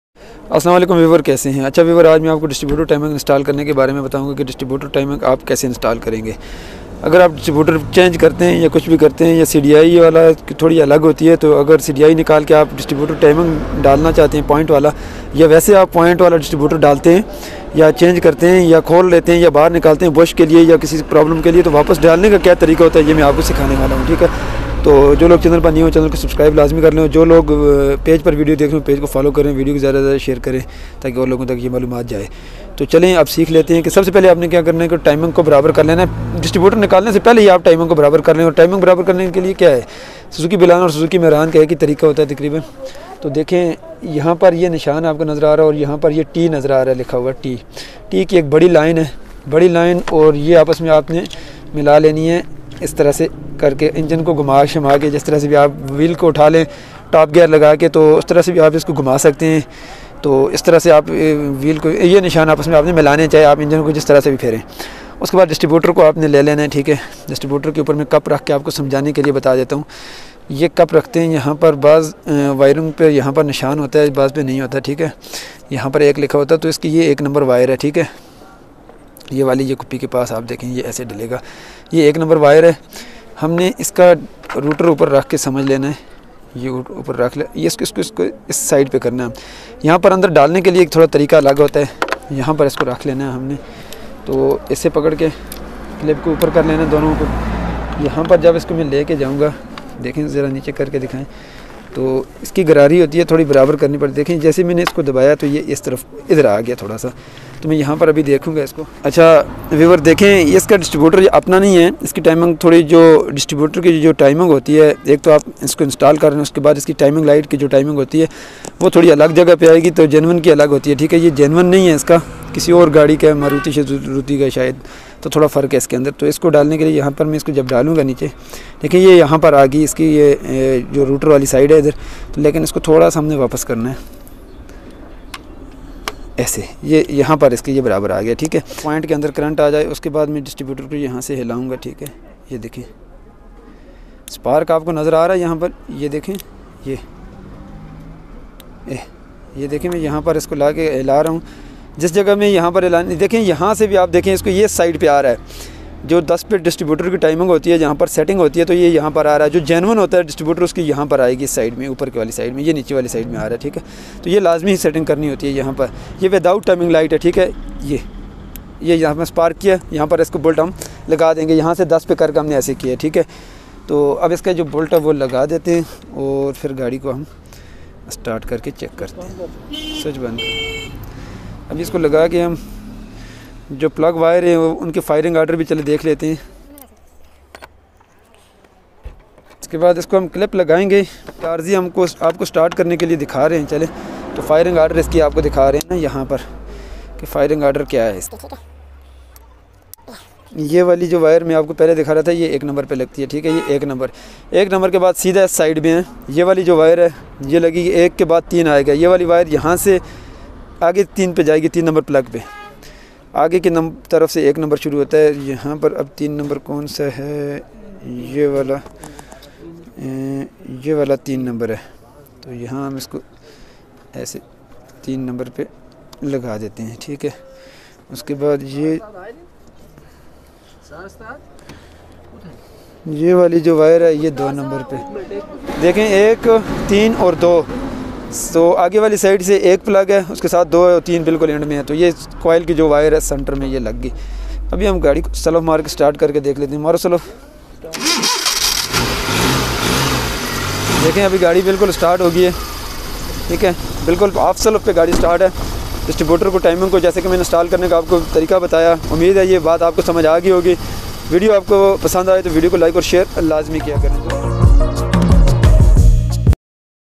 Distributer Installation F8B Engine | Ignition Timing Suzuki Bolan Suzuki Mehran | ڈسٹریبیوٹر ٹائمنگ
In a spark ignition internal combustion engine, Ignition timing refers to the timing, relative to the current piston position and crankshaft angle, of the release of a spark in the combustion chamber near the end of the compression stroke.
Ignition timing is measured in degrees of crankshaft rotation, before top dead center (BTDC). In a perfect world, The spark plug fires. The flame travels through the combustion chamber, igniting the air/fuel mixture.